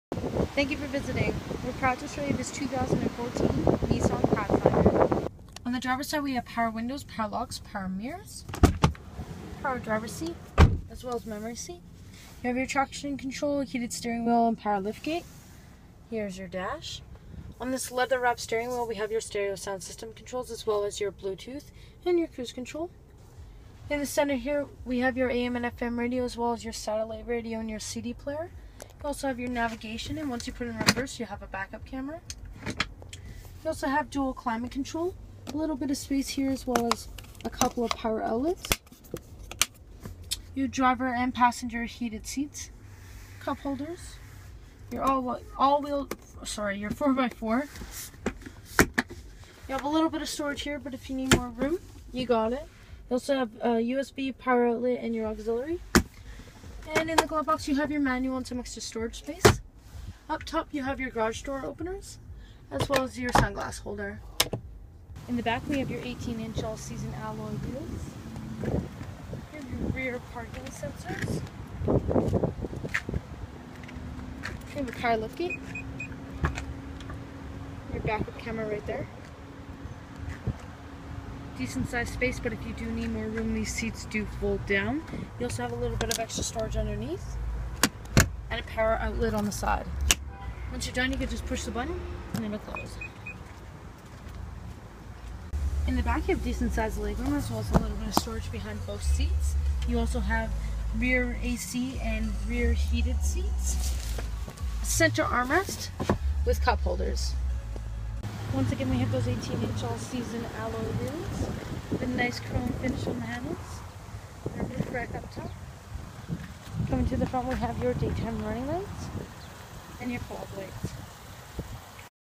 2014 Nissan Pathfinder 4WD SL | West End Nissan
This is a  2014 Nissan Pathfinder 4WD 4dr SL with CVT Transmission transmission White[QAA,Moonlight White Pearl] color and Black interior color.

West End Nissan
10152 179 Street
Edmonton, Alberta
T5S 1S1